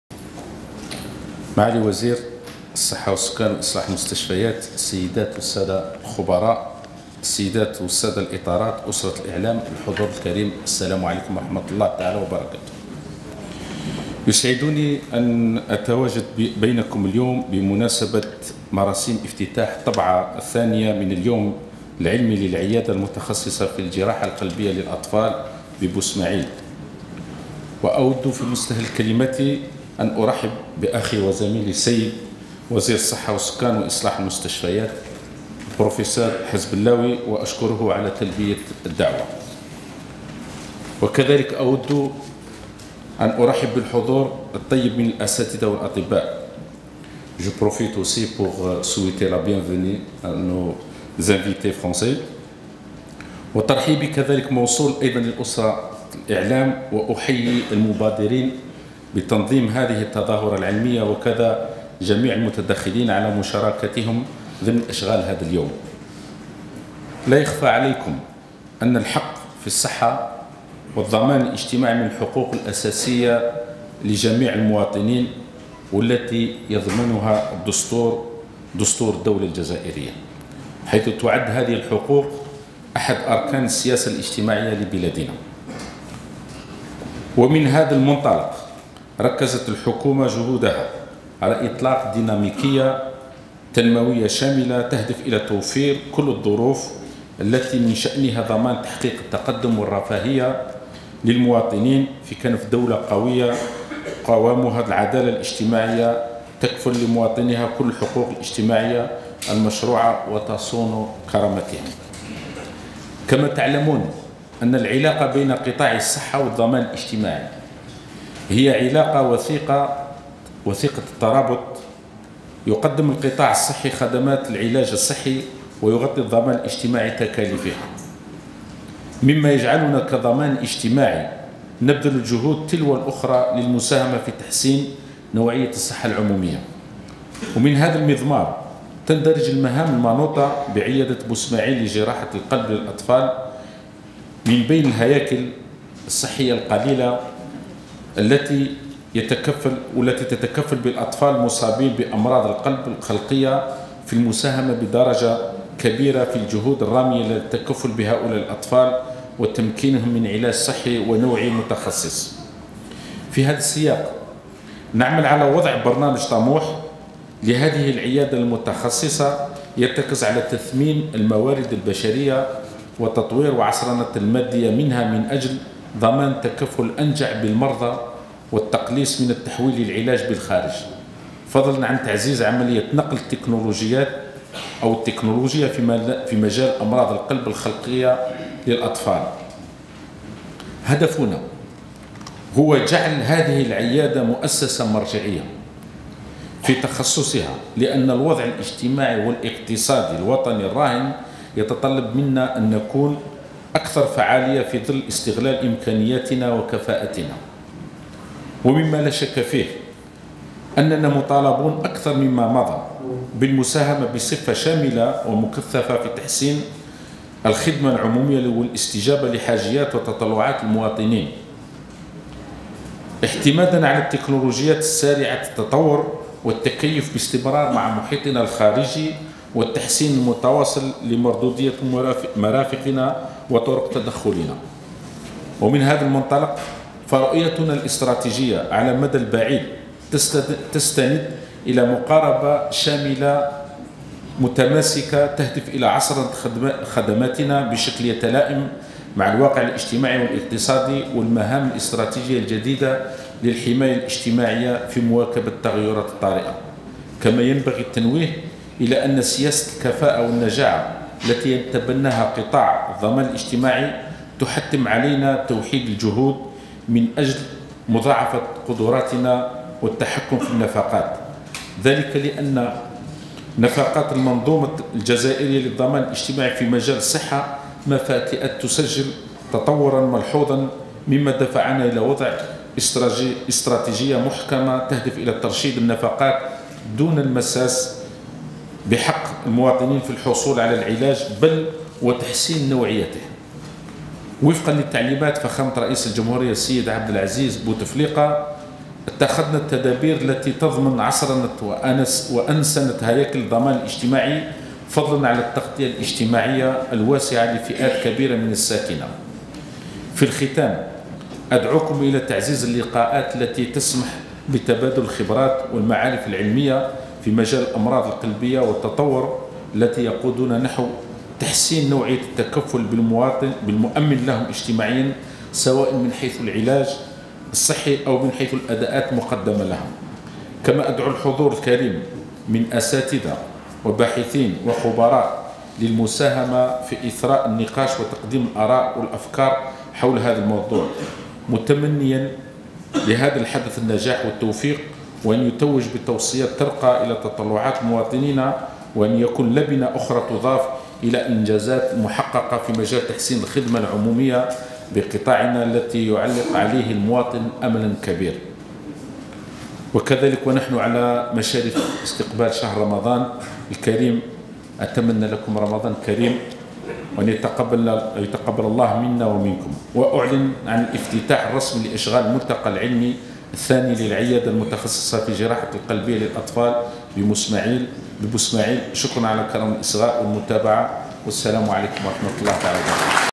كلمة وزير العمل السيد مراد زمالي / اليوم العلمي الثاني حول عيادة الجراحة القلبية للأطفال
أشرف وزير العمل والتشغيل والضمان الاجتماعي السيد مراد زمالي رفقة وزير الصحة والسكان وإصلاح المستشفيات، السيد مختار حسبلاوي، اليوم الإثنين 14 ماي 2018 على افتتاح أشغال اليوم العلمي الثاني حول عيادة الجراحة القلبية للأطفال ببواسماعيل (تيبازة).
السيد الوزير ذكر في كلمة ألقاها بالمناسبة بالمهام المنوطة بعيادة بوسماعيل لجراحة القلب للأطفال، مبرزا أن العيادة تعد الهيكل الصحي العمومي الوحيد الذي يتكفل بالأطفال المصابين بأمراض القلب الخلقية في المساهمة بدرجة كبيرة في الجهود الرامية إلى التكفل بهؤلاء الأطفال، وتمكينهم من علاج صحي نوعي متخصص.
وأضاف السيد مراد زمالي أن العلاقة بين قطاعي الصحة والضمان الاجتماعي هي علاقة وثيقة الترابط، إذ يقدم القطاع الصحي خدمات العلاج الصحي، ويغطي الضمان الاجتماعي تكاليفها.

وزارة العمل والتشغيل والضمان الاجتماعي - الجزائر